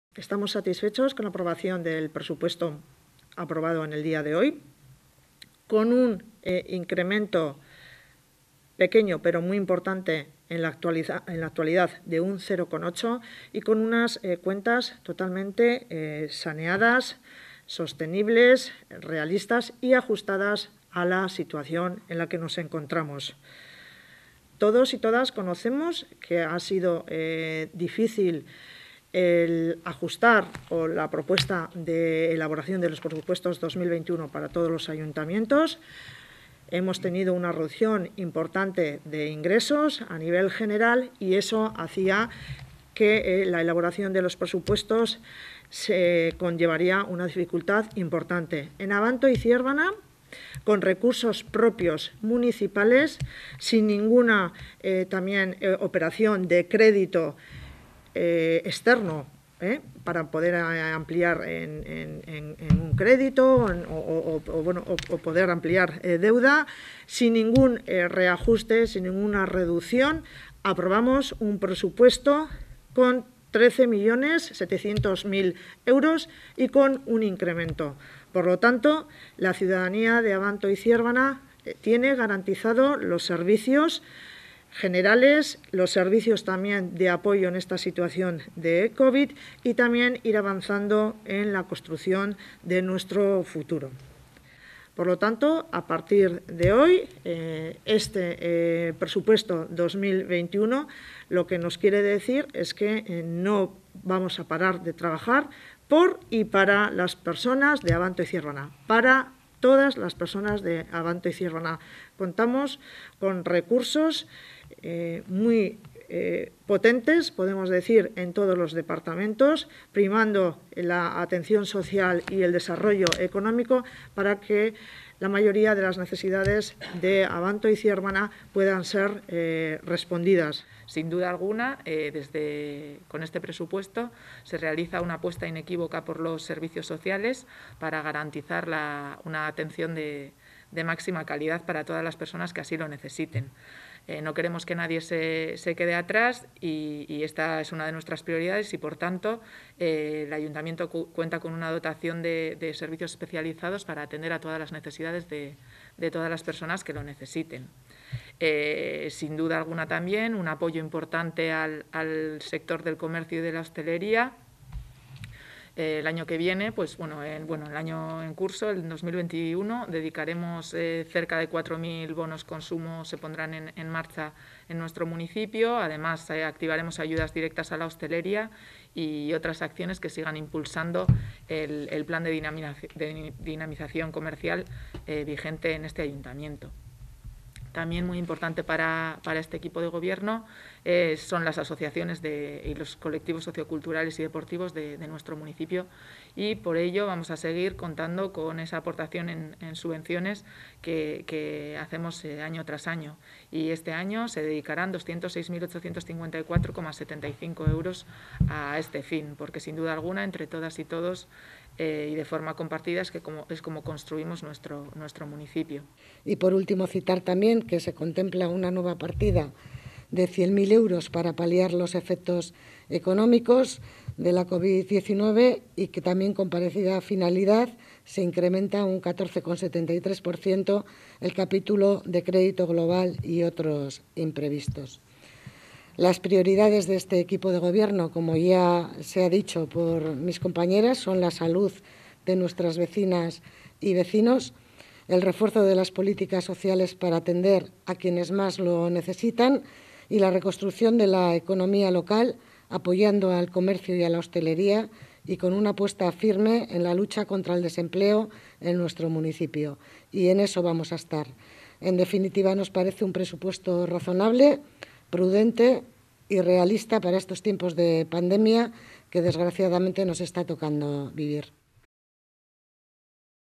Abanto aprueba un presupuesto sin restricciones
Es más, pese a las actuales circunstancias,  las cuentas municipales se incrementan un 0,8 % hasta superar los 13, 7 millones de euros.

El Ayuntamiento prioriza en sus presupuestos el bienestar de la ciudadanía, el apoyo al tejido económico local y el desarrollo futuro del municipio.